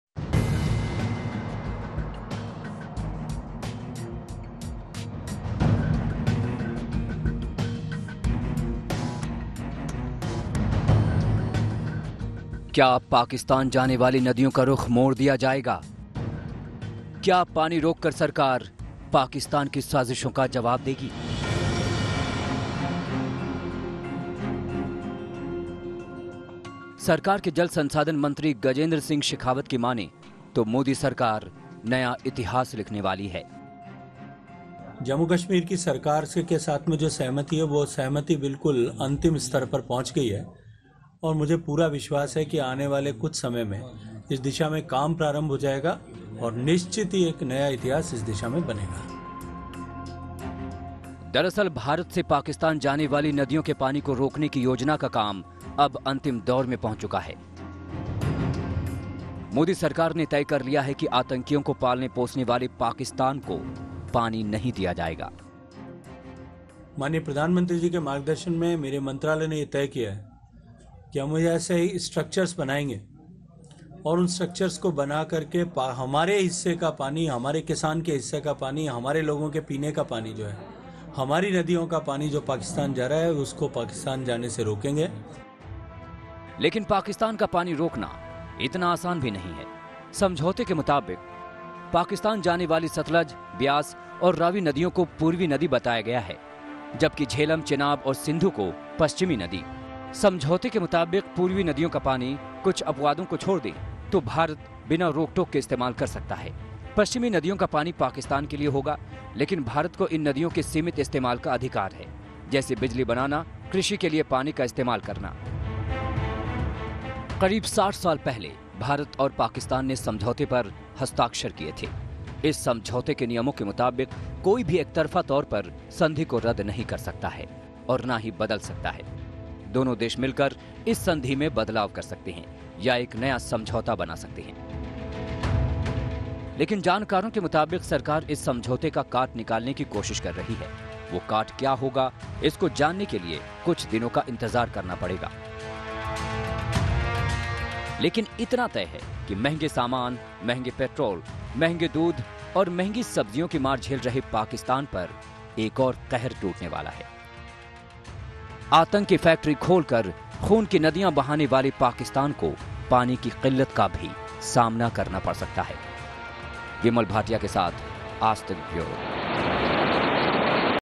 Modi Govt बंद करने जा रही PAK का पानी, जल्द आएगा बड़ा फैसला !
भारत ने पाकिस्तान को सबक सिखाने के लिए तैयारी तेज कर दी है. भारत की नदियों का पानी भारत को मिलना जल्द ही बंद हो जाएगा.  केंद्रीय जल शक्ति मंत्री गजेंद्र सिंह शेखावत ने कहा कि भारत से पाकिस्तान जाने वाले नदियों के पानी को पाकिस्तान में जाने से रोकने की योजना का काम अब अंतिम दौर पर पहुंच चुका हैं.